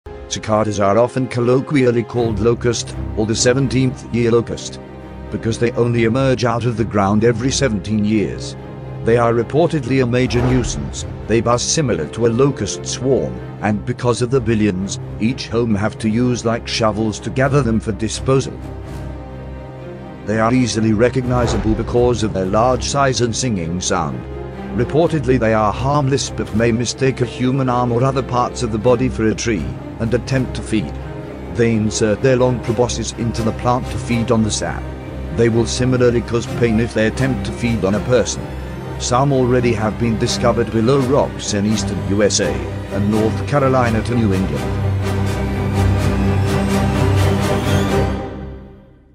USA LOCUST / CICADA INVASION!! Billions ARRIVE! NJ NY CN VG CN NK 5.6.13
UPDATE:
New Jersey
- 5.1.13 Resident in Short Hills say "The cicadas are beginning to dig mud tunnels. Ugh."
New York:
- 4.26.13 Resident in New Windsor say: "I pulled back a tarp had laying down all winter long and found more 3/4" holes in the ground than I can count, including emerging cicadas".
Virginia:
- 4.11.13 Resident of Dale City says: "I noticed cicada holes all over Dale City and Woodbridge Virginia today that seemed to pop up over night"
- 4.11.13 Resident of Virginia Beach: "I hear the unmistakable sound of cicadas. Just happened today"

According to scientists, locust like Cicadas will appear on the East coast of the USA in their 17 year cycle. It is unwise to dismiss it as only a cycle. God uses storms, snow, rain etc to appeal to man's conscience though they are somewhat cyclical sometimes even yearly.
Amos 4:10 I have sent among you the PESTILENCE after the manner of EGYPT(LOCUST 2013): ...yet have ye NOT RETURNED unto me, saith the LORD.DON'T BREAKDOWN BEFORE YOUR BREAKTHROUGH

ABOUT CICADAS
- ",,often colloquially called l-o-c-u-s-t-s" (Wikipedia)
- Called the 17th year Locust
- Major Nuisance
- Buzz similar to Locust Swarm
- Because of the billions, each home have to use like shovels to gather them for disposal
- Recognized easily becuase of large size & singing sound
- Reportedly harmless but may mistake a human arm or other parts of the body for a tree and attempt to feed (Wikepedia)
- They insert their long proboscis into the plant to feed on sap
- They similarly cause pain if they attempt to feed on a person
- Can damage crops, shrubs 7 trees
- They will scar the trees while the females lay eggs deep in branches
- Like Locust, many people eat them: Greece, China, Malaysia, Burma, Latin America, Congo
- Some already dicovered below rocks in Eastern USA
- North Carolina to New England
- Emerge @ 64 Degrees
- Though 8 inches below they will arise
- Eatenbby birds and Sqirrels
- They will
o Climb Trees
o Shed exoskeletons
o Sing
o Fly
o Mate
o Lay Eggs
o Die
- Harmless except for the mess and loud mating calls
- Loud chorus of sing overwhelm whole neighborhoods
- Piles of dead insects will be found around the homes

NEWS ARTICLES:
Otherwise seen in the Carolinas and Connecticut